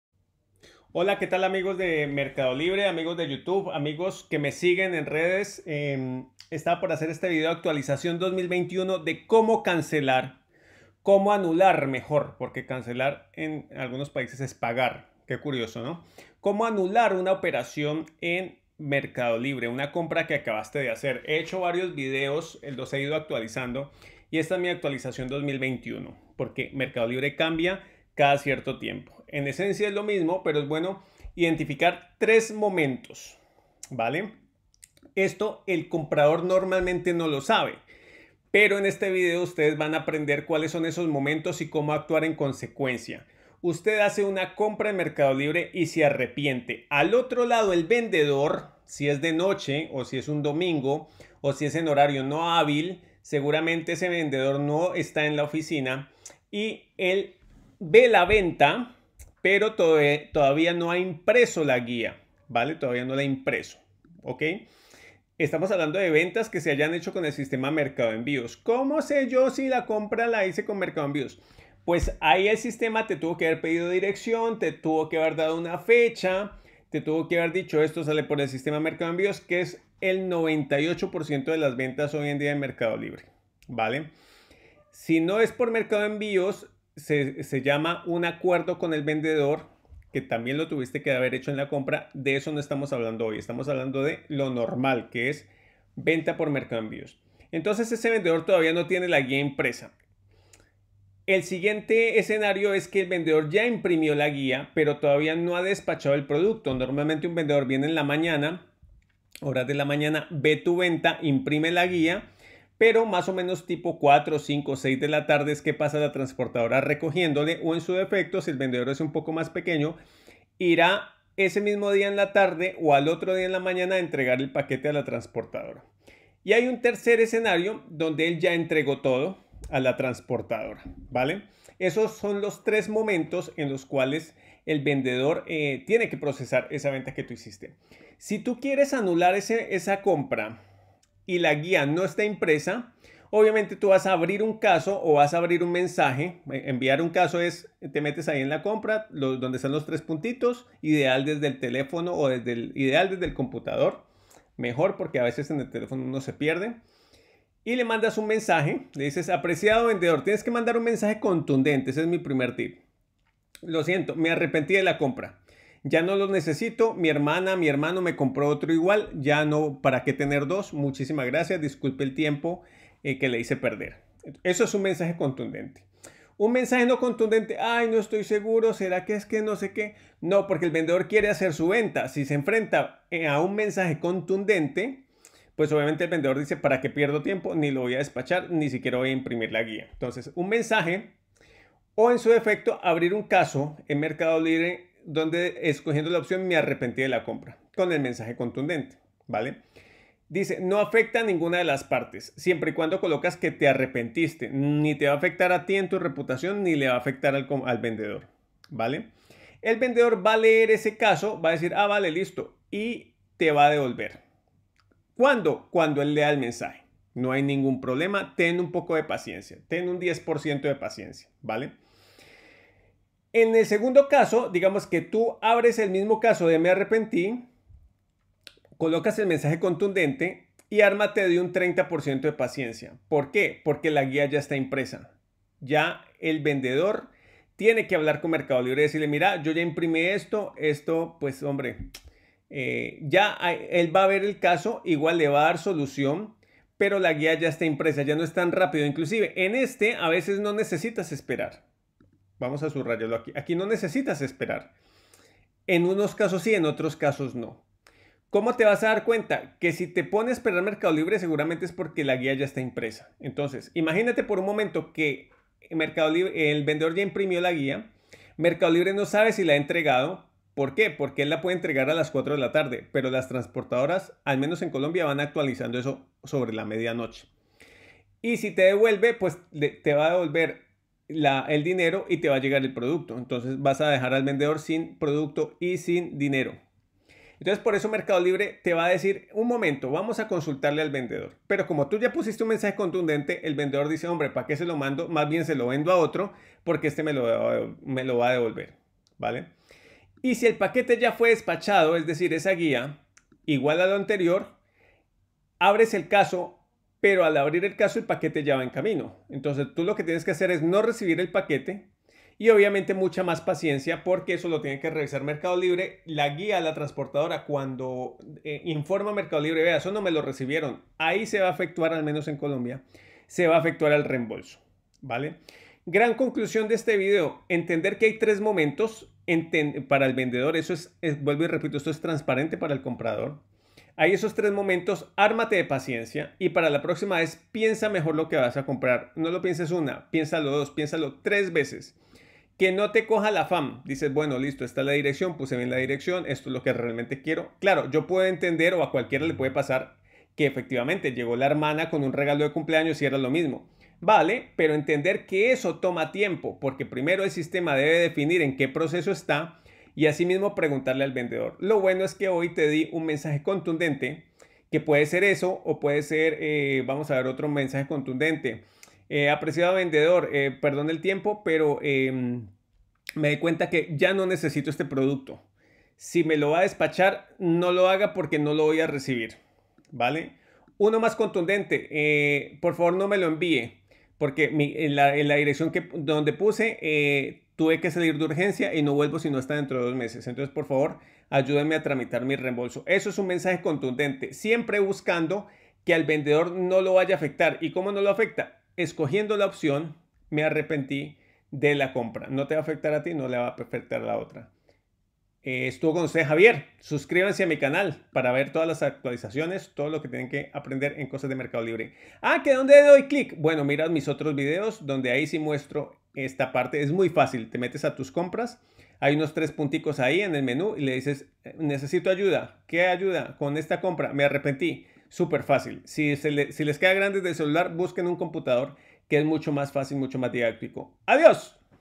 Act 2021 Anular / Cancelar compra en MercadoLibre Actualizado 2021
Ya era hora de este video. A armarse de paciencia al anular una compra.